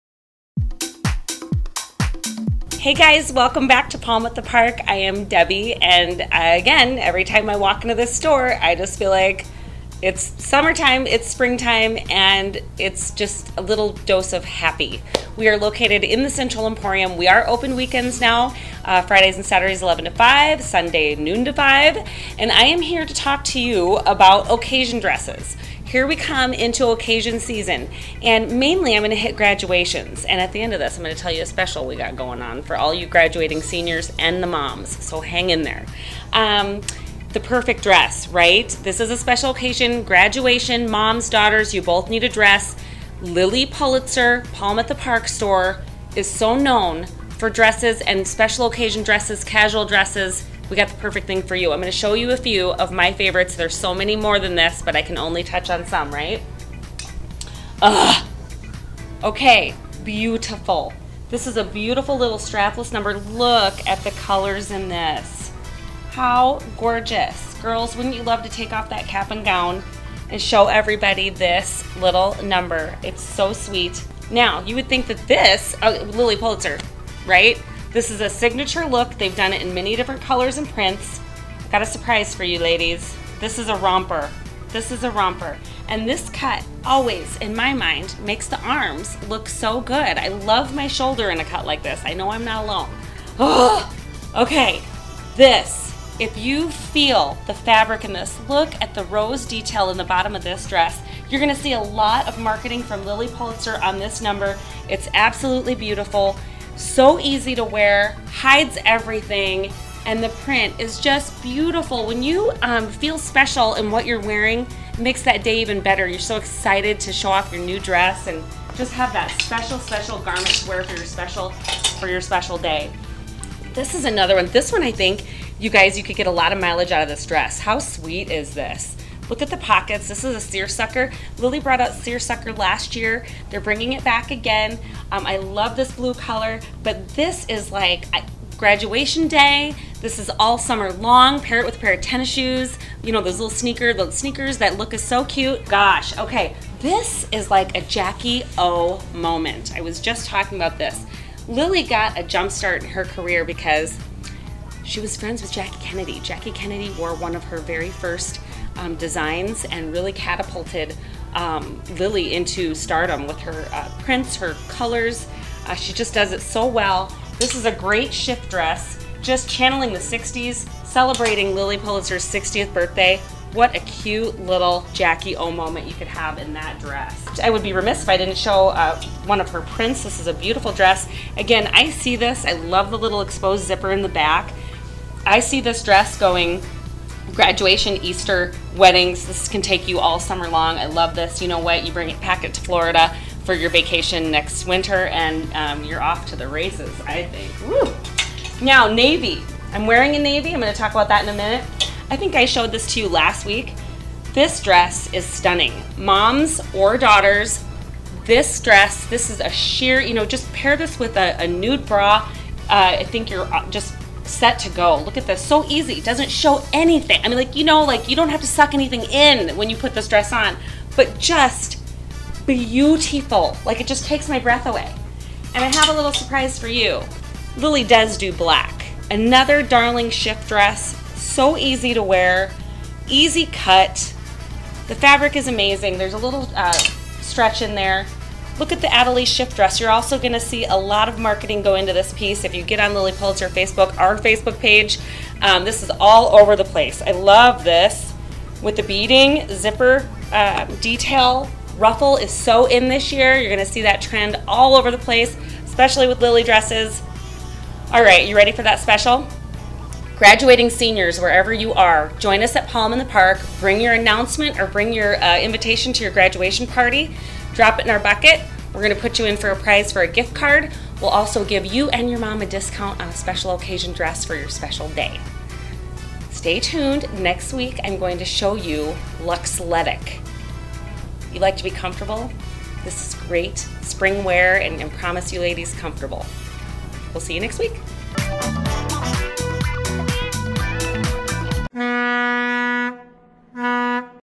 Marina District TV : Graduation dresses at Palm at the Park
Every upcoming special occasion - Palm at the Park and Lilly Pulitzer have the perfect style and fit.